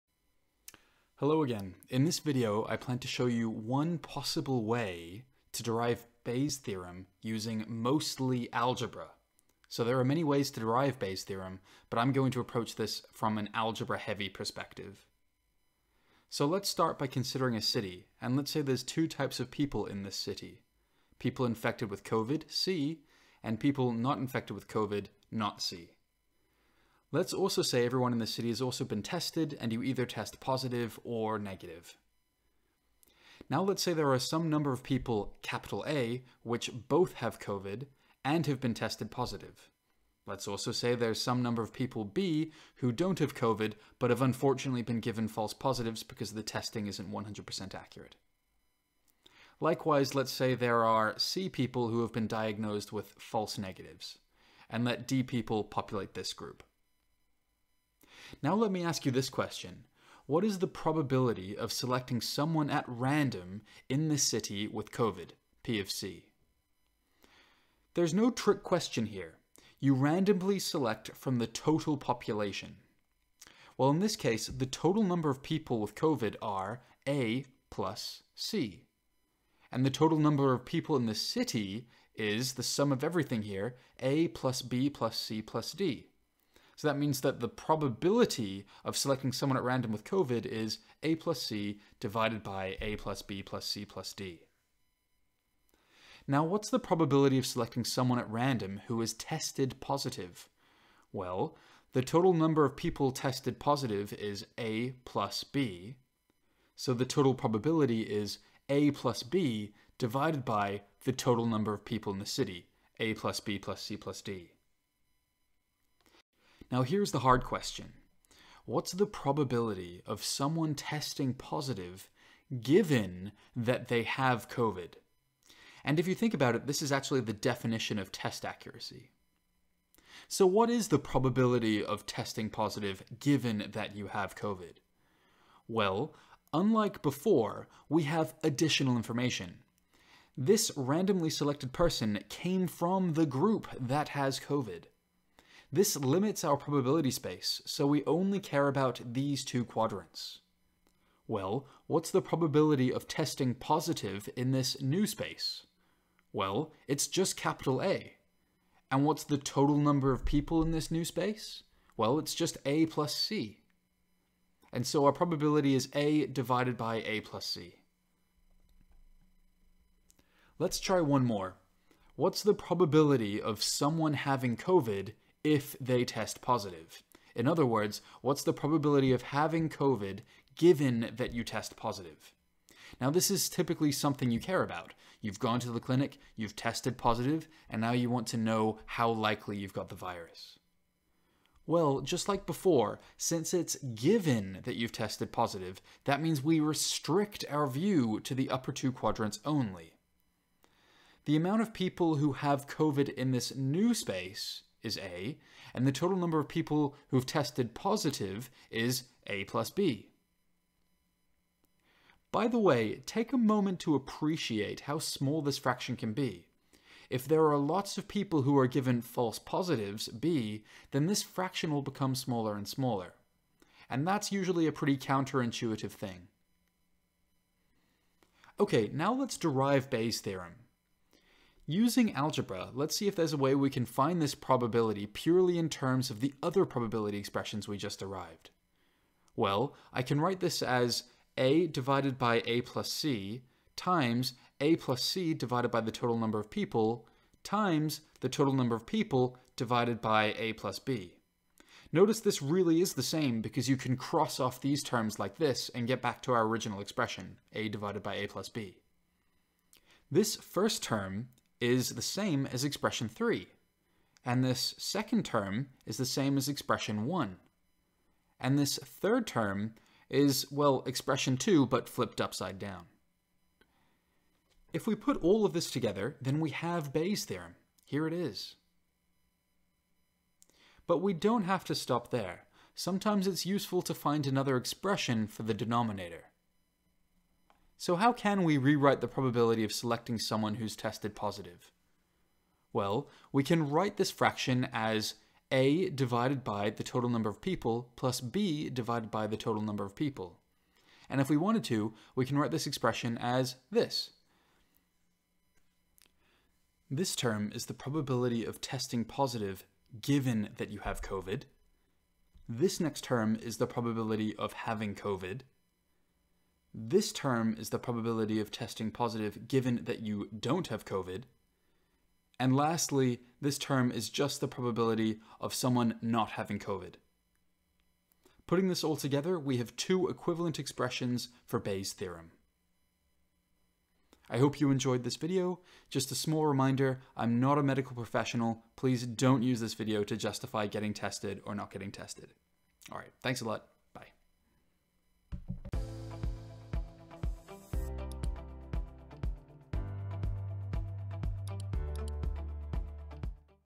Bayes' theorem alternative proof (using algebra)
There are many ways to prove Bayes' theorem. This is an alternative proof which is more algebra heavy and doesn't rely on a strong knowledge of conditional probability.

I am an Engineer, not a medical professional. I chose the topic of Covid because it's a fantastic example to teach Bayes' theorem. Do not use this video for medical advice.